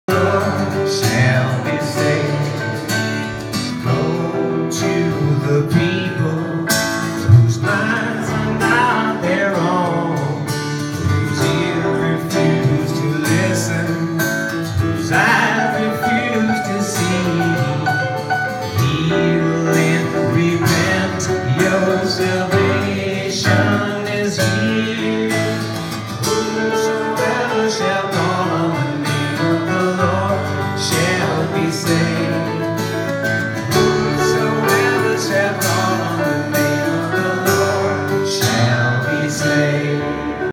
Fik!
St. Clare of Montefalco Discipleship and mission lecture series. :40 second clip of a two man duo that was rocking the roof off of St. Philomena parish, on Thursday night, April 19, 2018. This is :40 seconds of Heaven on Earth!